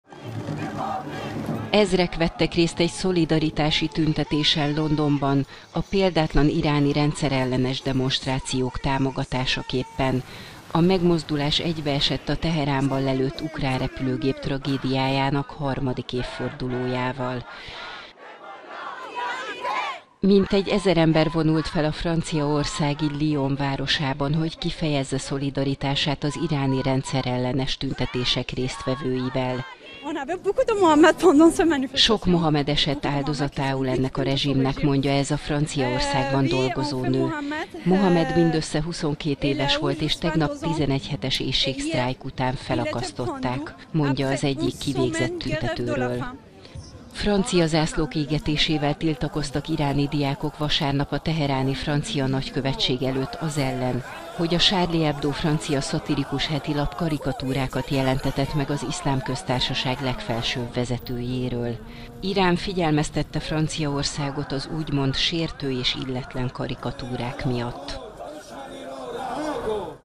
Karikatúrákat jelentetett meg a Charlie Hebdo az iráni vezetőről
Irán figyelmeztette Franciaországot a "sértő és illetlen" karikatúrák miatt, amelyek a Charlie Hebdo különkiadásában jelentek meg a magazin párizsi irodái elleni 2015-ös halálos támadás évfordulója alkalmából.